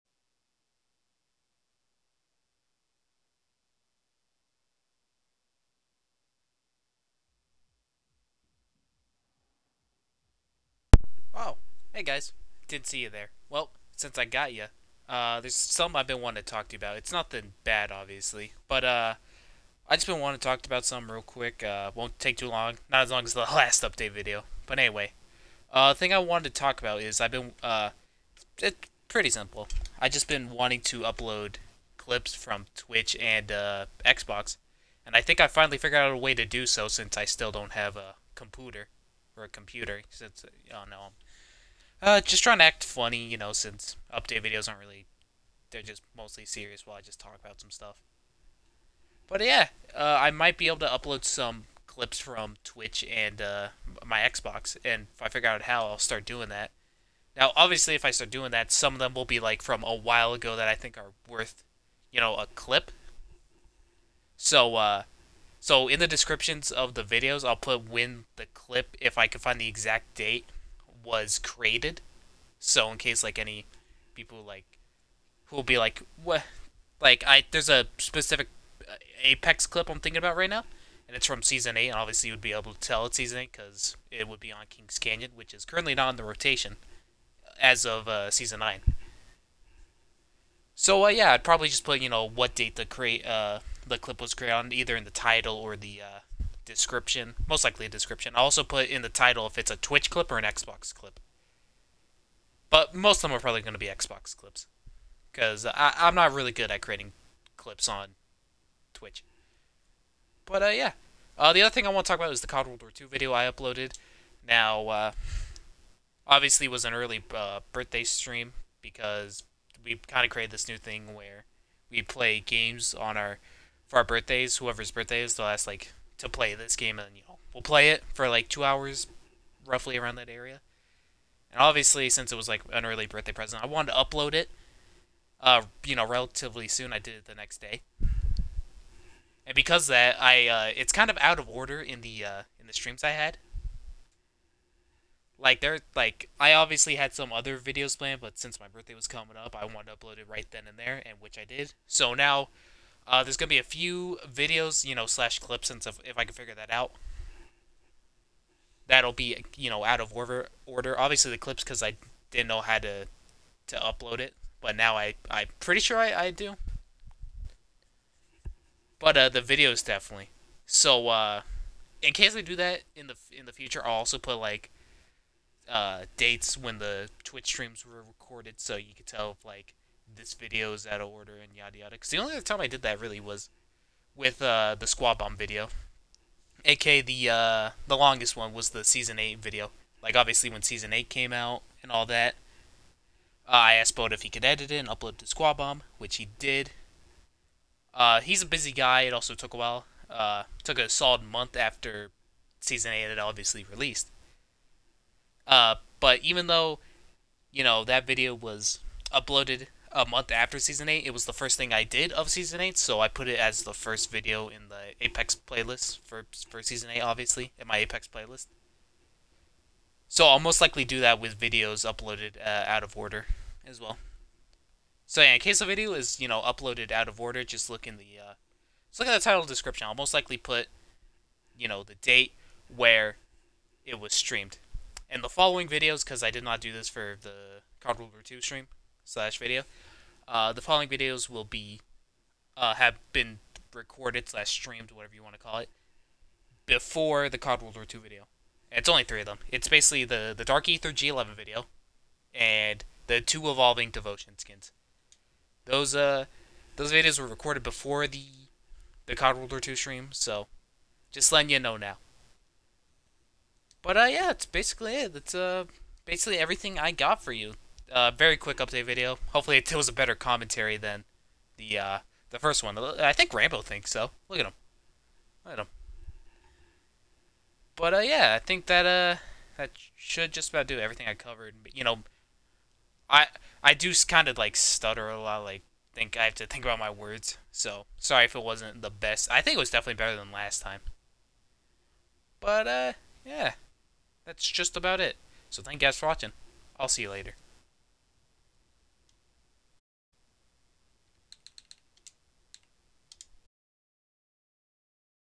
Update Video 6/16/21
In this update video I talk about how I wanna start uploading Twitch clips and Xbox clips to the channel and how some videos get streamed and uploaded before other videos.

What are you doing down here?